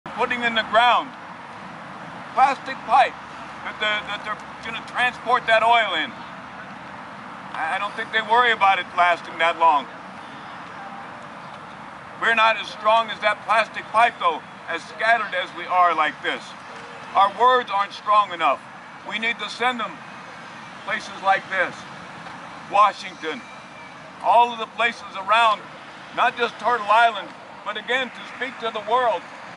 Dakota Access Pipeline Resistance 1
Native leader speaking about the Dakota Access Pipeline in Indianapolis